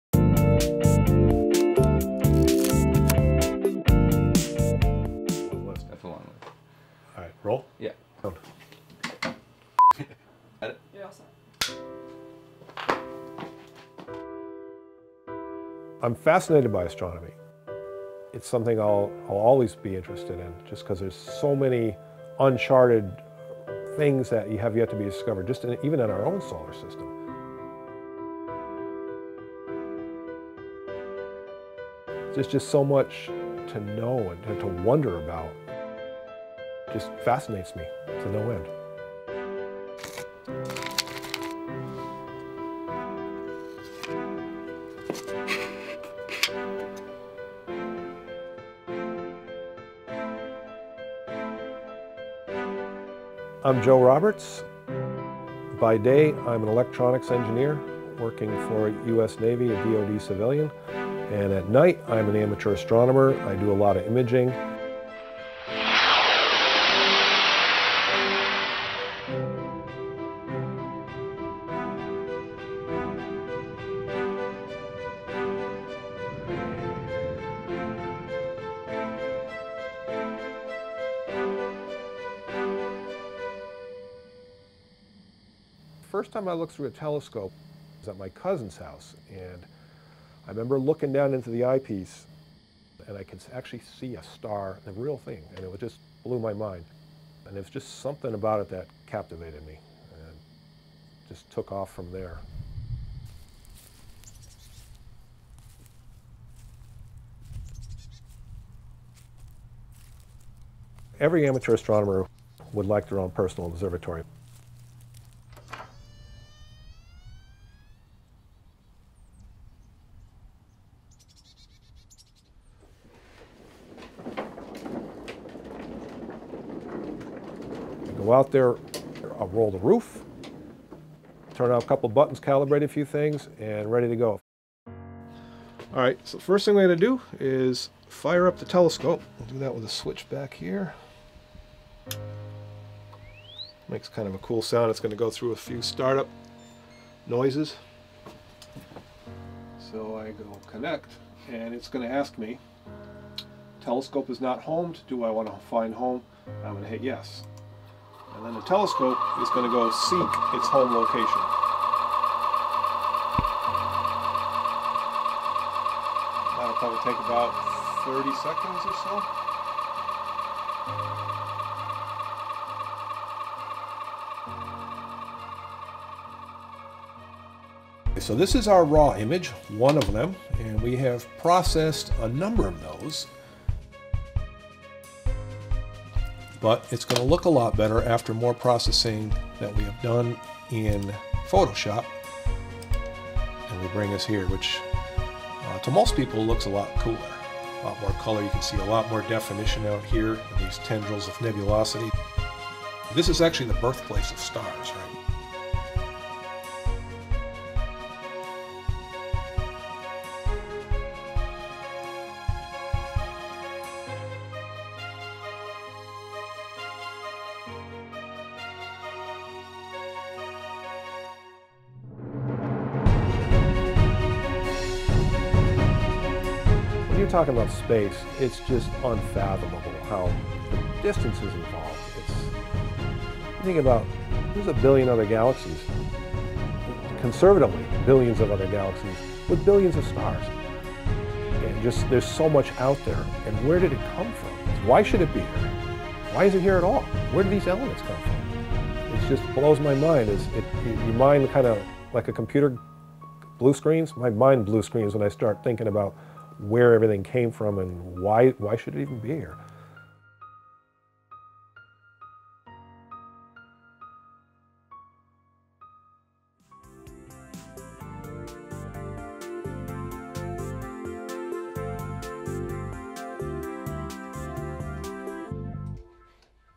This Amateur Astronomer Built an Observatory in His Backyard | American Lens | NBCLX
Joe Roberts spends his days as an electronics engineer working for the U.S. Navy and Department of Defense. But his passion is exploring the cosmos every night from the sophisticated observatory he built in a shed in his own backyard.